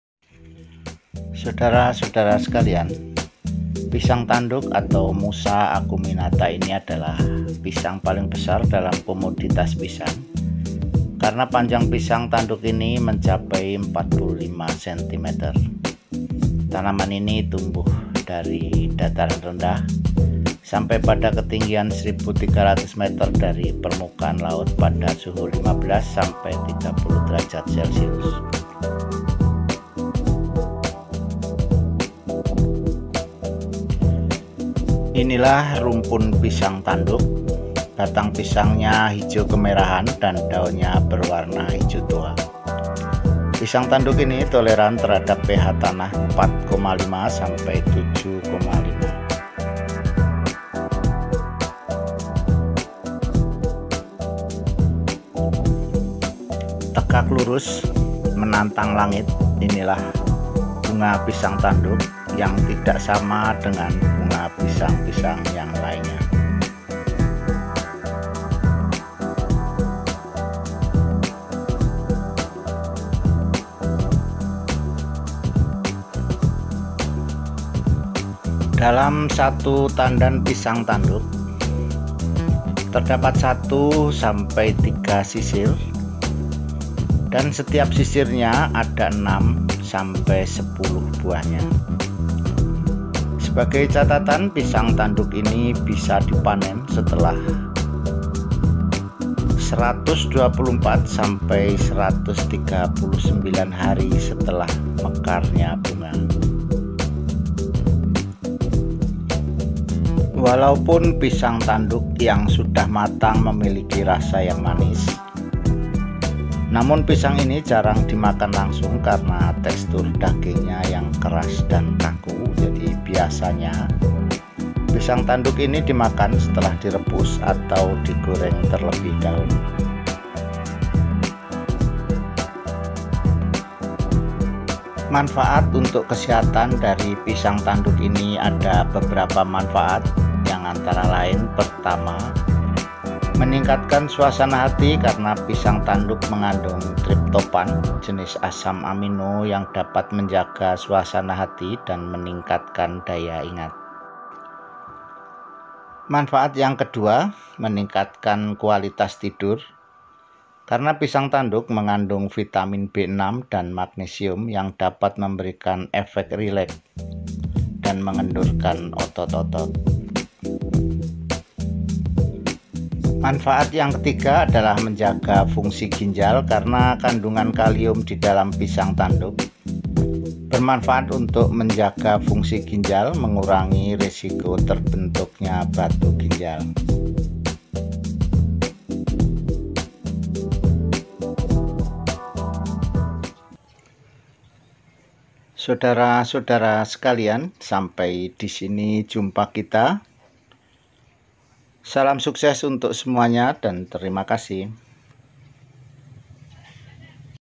"PISANG TANDUK" meningkatkan kuwalitas tidur,@Ushter studio.
Pisang tanduk selain meningkatkan suasana hati dan daya ingat,pisang tanduk bisa juga meningkatkan kuwalitas tidur.
             UIN Alauddin Makasar(Sutrisna S)